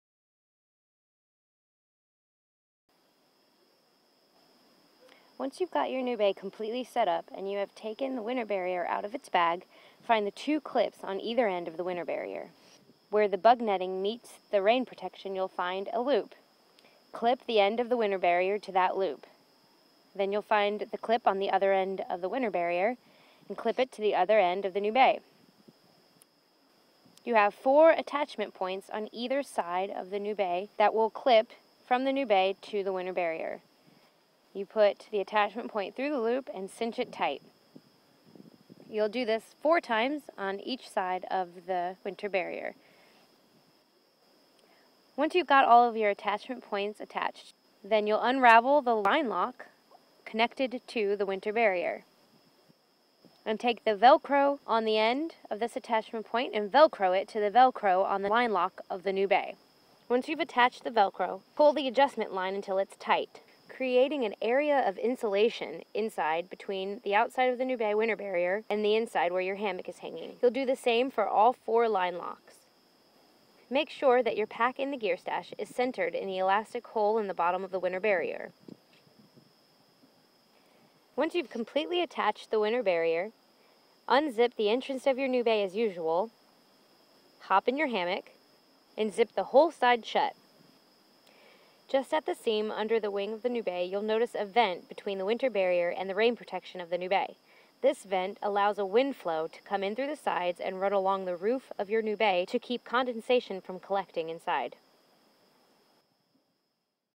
Nubé Winter Barrier: How to Set Up
The Nubé Winter Barrier is designed as an add-on layer of protection for the Nubé when used in the colder months of the year. The Winter Barrier attaches to the exterior of your Nubé mosquito netting and seals out the bitter cold wind, thus reducing convection heat loss from your body. The Winter Barrier attaches with existing clips on the Nubé and seals snugly around your mosquito netting with it's integrated synch systems. The Winter Barrier does not prevent the use of the Gear Storage area on the Nubé. It is constructed out of a breathable nylon ripstop in order to reduce condensation build up in the system and also has breather ports built in reducing condensation further.